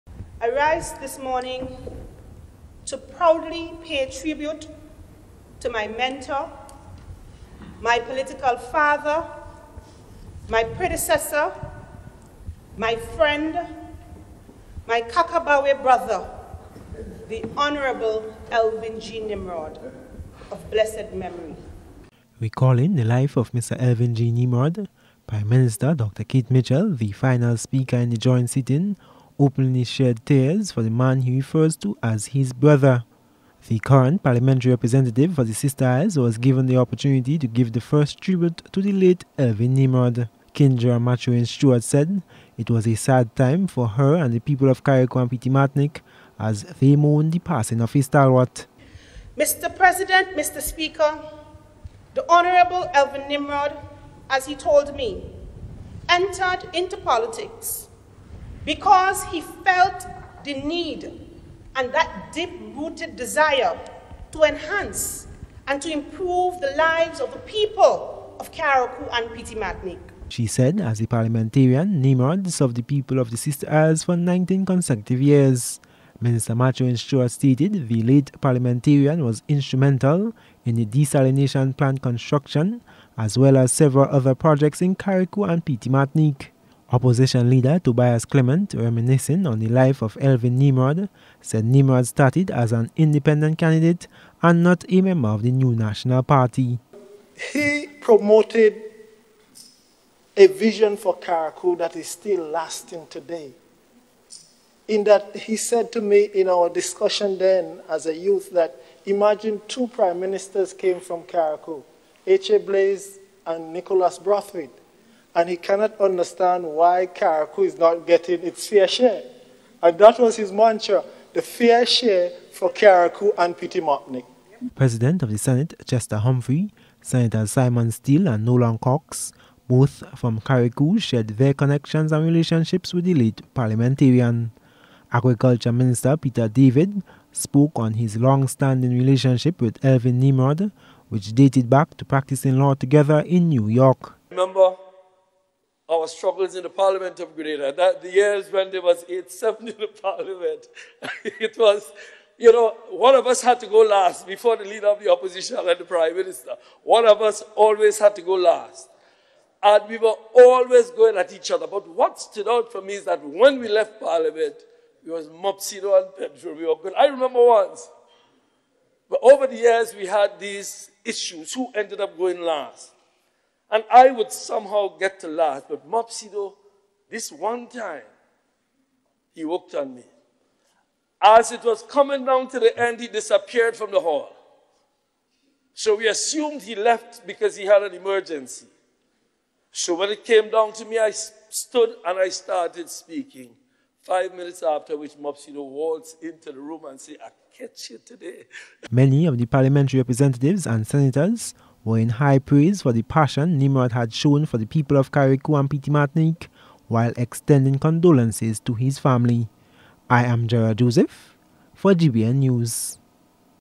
Elvin Nimrod Remembered
A motion was passed to recognize the substantial contributions made by the late Elvin Nimrod as a resolute parliamentarian. Prime minister Dr. Keith Mitchell on moving the motion in the special sitting of both houses of parliament on Tuesday expressed sympathy to the family, relatives, and friends of Elvin Nimrod.